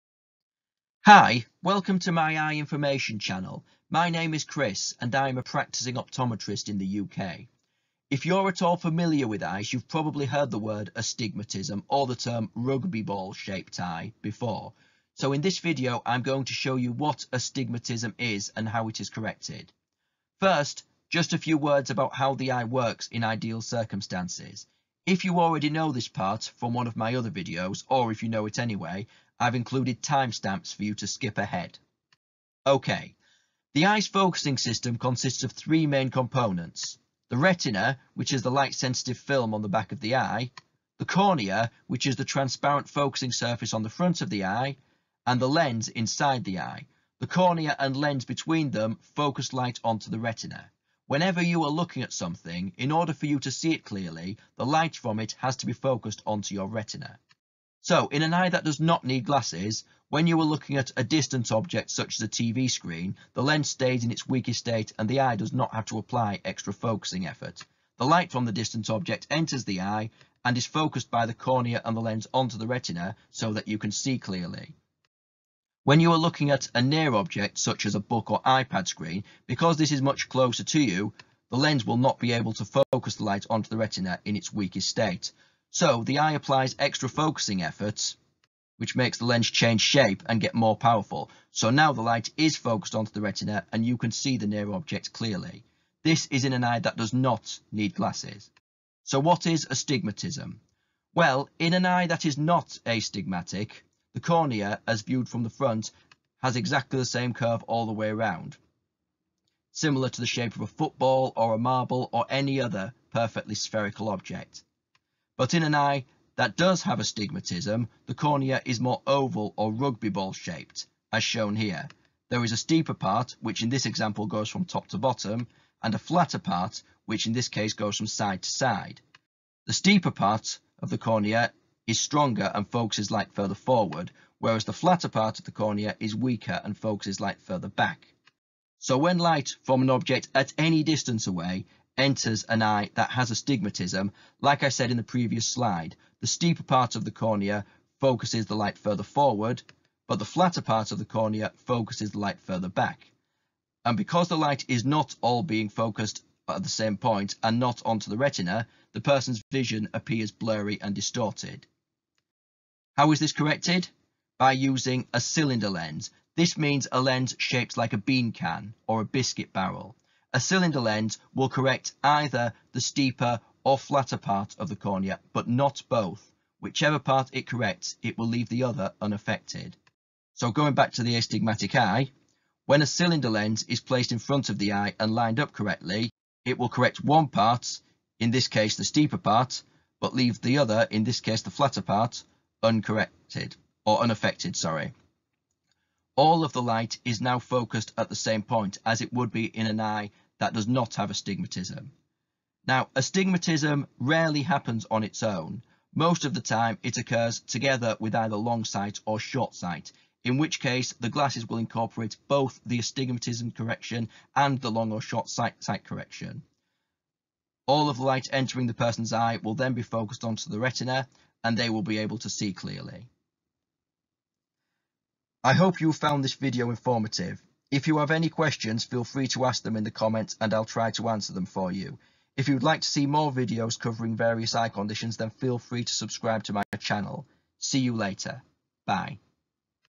Astigmatism explained
Hi. Here is a simple video explaining all about astigmatism.

0.00 - Intro & how the eye works.
1.38 - What is astigmatism?
2.42 - Astigmatism correction.

Chris